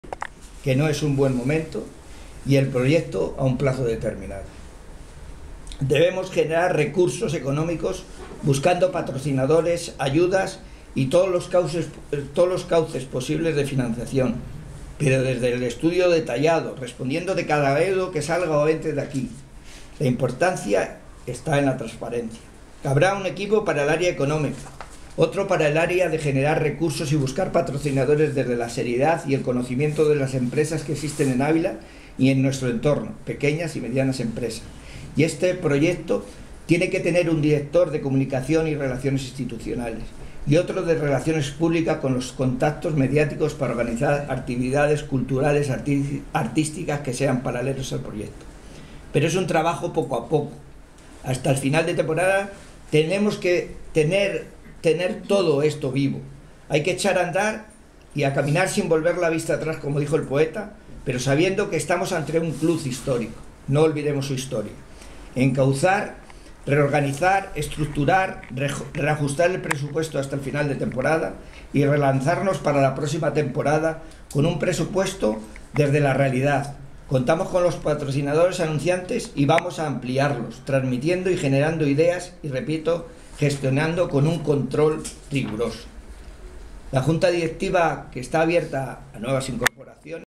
Nuevo Presidente Real Ávila III
Presentación del proyecto del nuevo presidente del Real Ávila, Fernando López del Barrio, en el estadio municipal Adolfo Suárez.